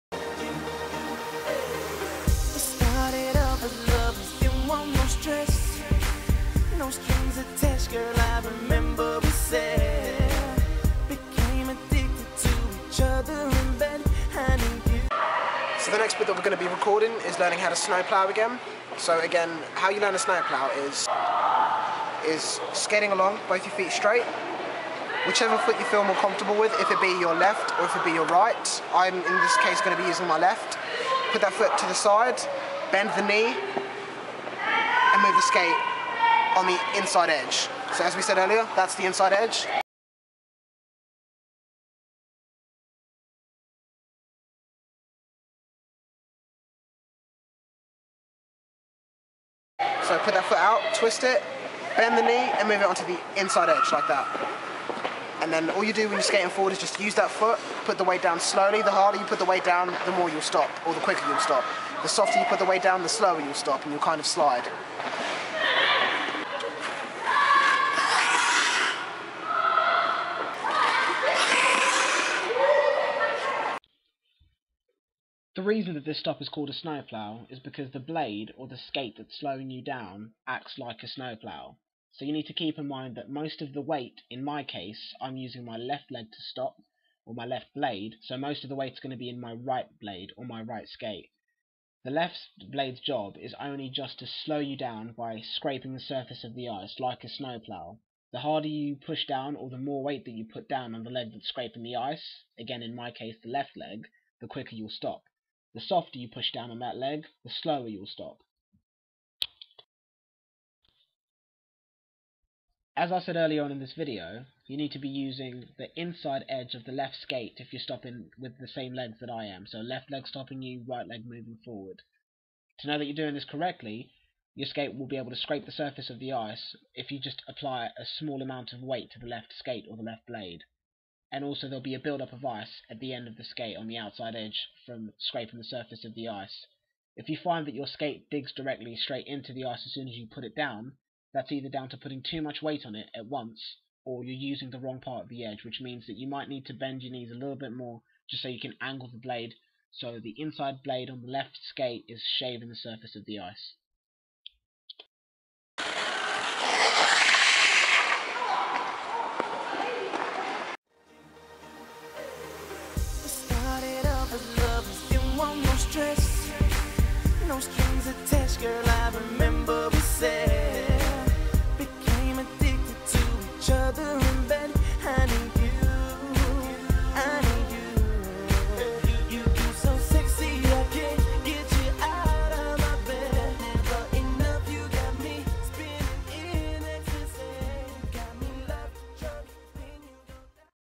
Snow Plough - How To Do A Basic Stop On Ice While Skating SnowPlow For Beginners
This video shows you how to do a basic stop on the ice while skating.